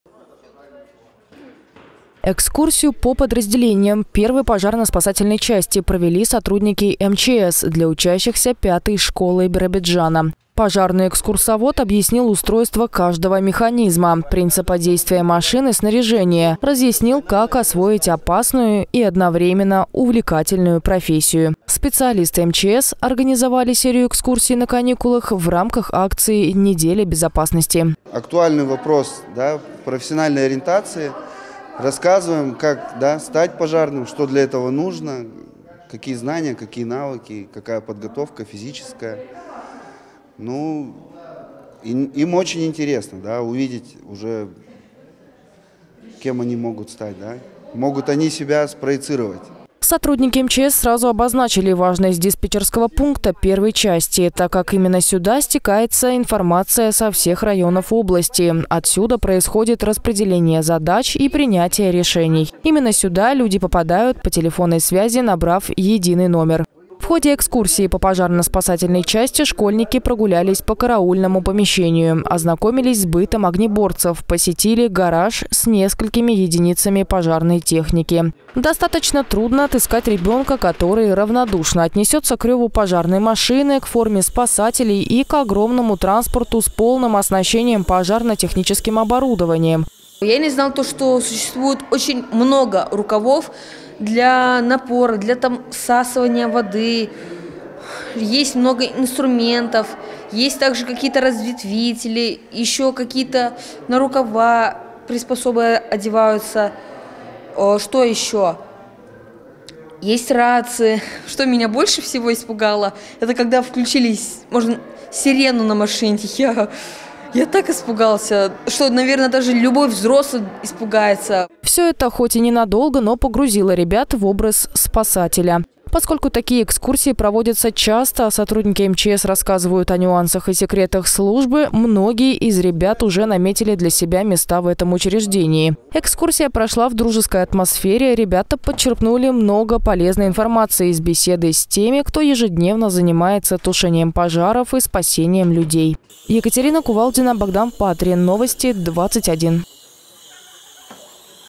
На экскурсии в пожарно-спасательной части побывали школьники Биробиджана
Экскурсию по подразделениям первой пожарно-спасательной части провели сотрудники МЧС для учащихся пятой школы Биробиджана. Пожарный-экскурсовод объяснил устройство каждого механизма, принципа действия машин и снаряжения, разъяснил как освоить опасную и одновременно увлекательную профессию. Специалисты МЧС организовали серию экскурсий на каникулах в рамках акции «Неделя безопасности».